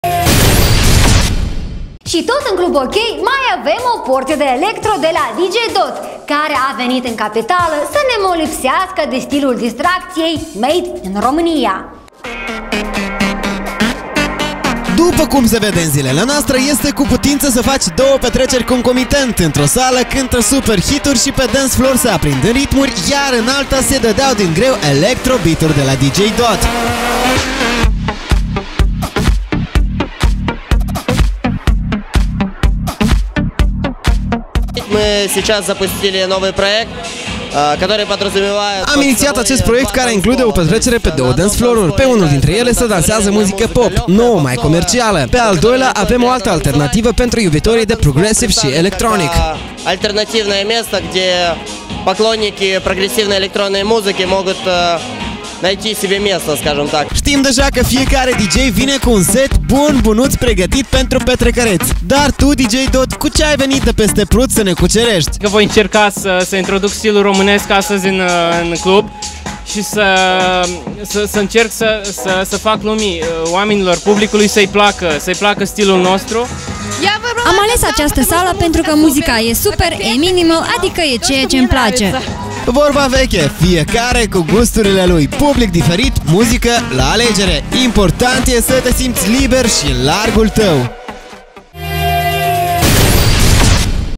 Stereo-Mono Project
Si tot in Club-OK mai avem o portie de elctro de la DJ Dot, care a venit in capitala sa ne molipseasca cu stilul de distractie "Made in Romania".